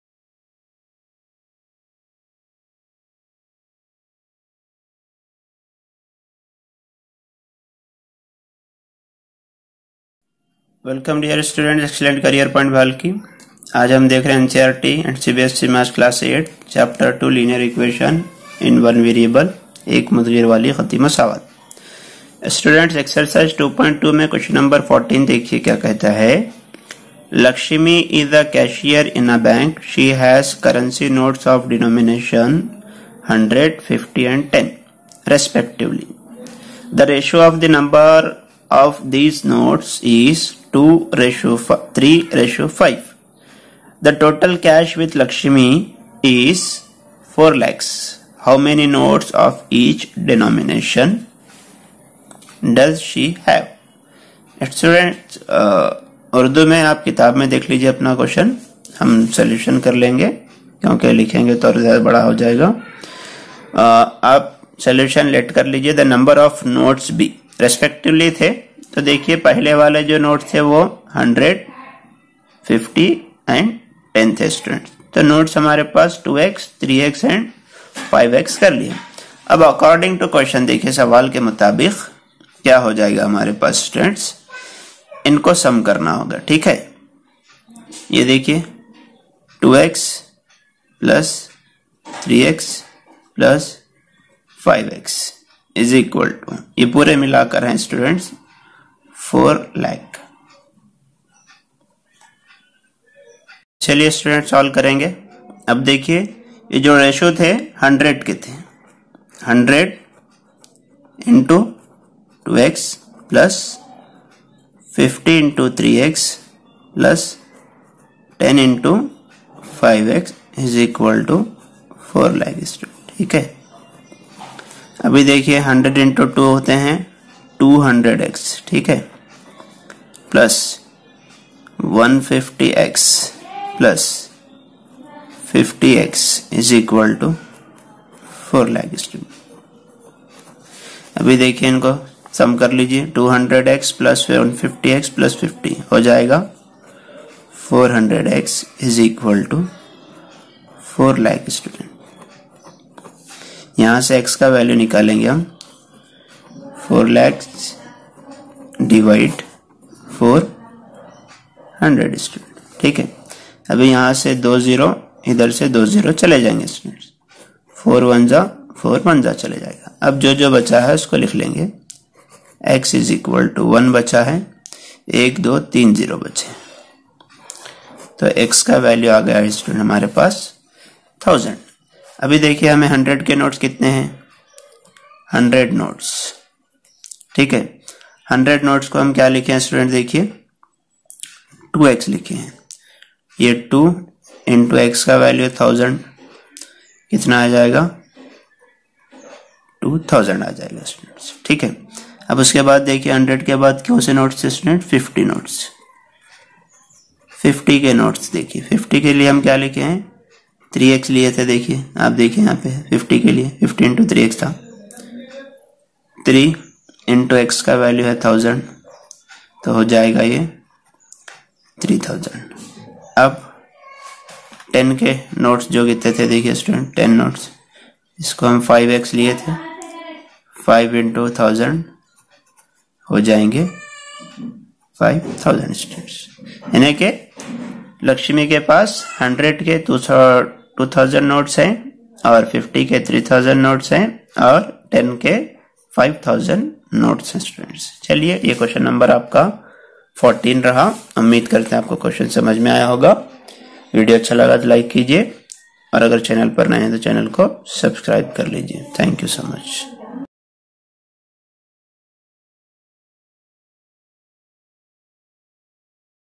Ex 2.2 Q14 - Maths Class 8 - Linear Equation in one variable - NCERT & CBSE - ایک متغیر والی خطی مسا
Ex 2.2 Q14 - Maths Class 8 - Linear Equation in one variable - NCERT & CBSE - ایک متغیر والی خطی مساوات
By Syed Asif Ali